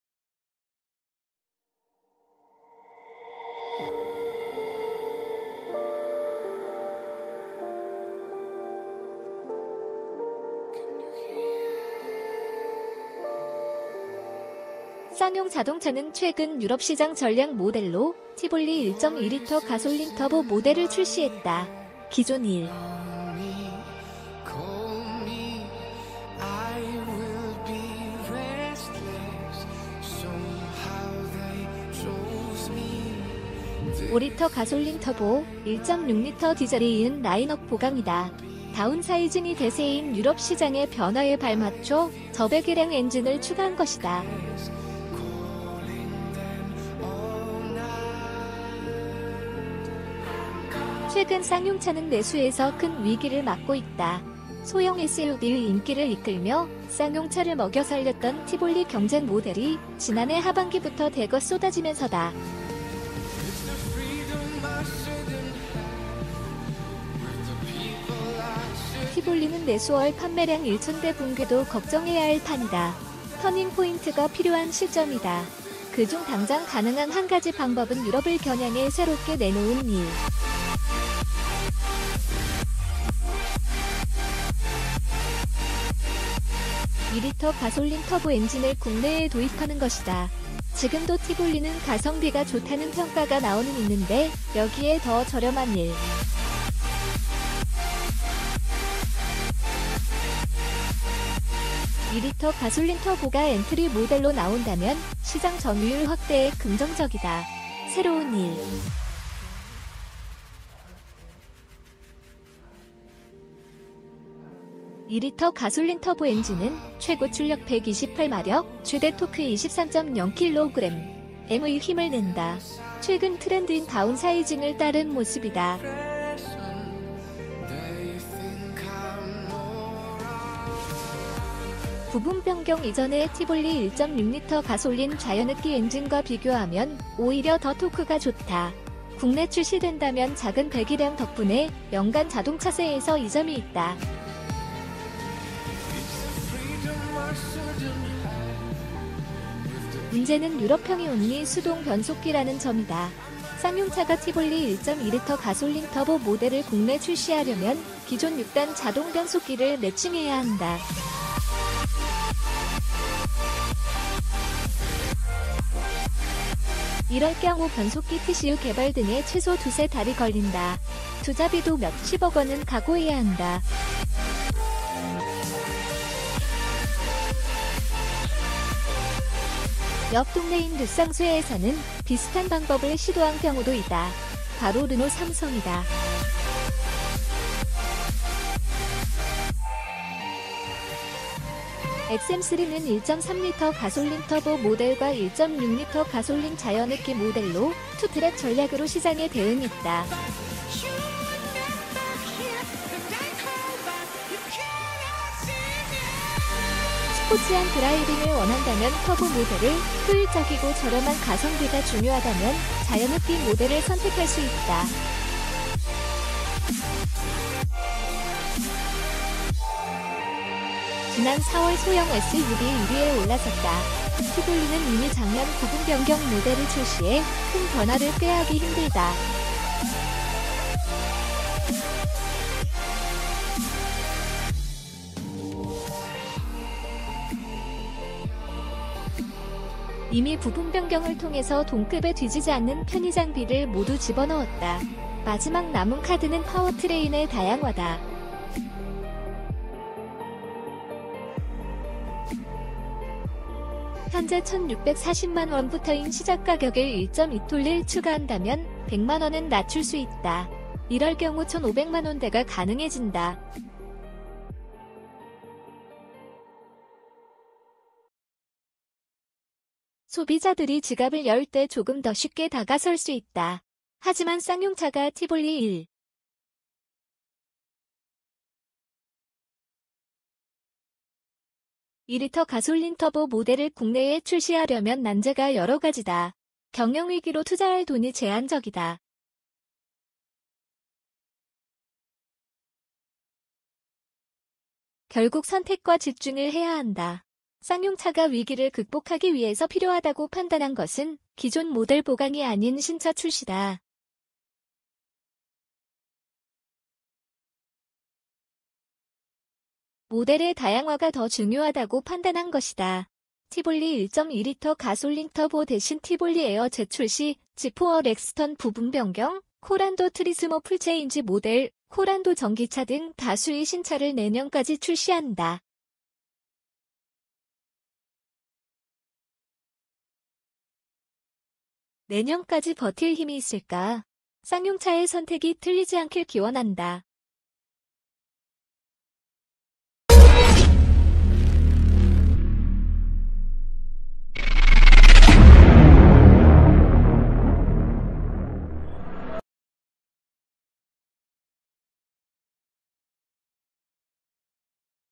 가성비 짱 티볼리 1.2T 가솔린..국내 도입할까?
가성비 짱 티볼리 1.2T 가솔린..국내 도입할까? 기존 1.5L 가솔린 터보, 1.6L 디젤에 이은 라인업 보강이다. 다운